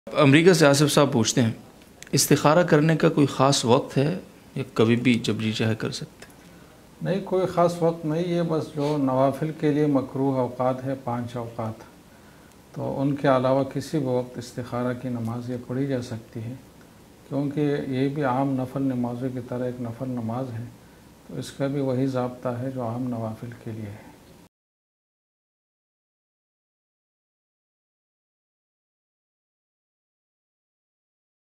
Istikhara Karne Ka Koi Khas Waqt He? | Jamia-Tur-Rasheed
Istikhara Karne Ka Koi Khas Waqt He?
Mufti Hussain Khalil KHail Sab
استخارہ کرنے کاکوئی وقت ہے؟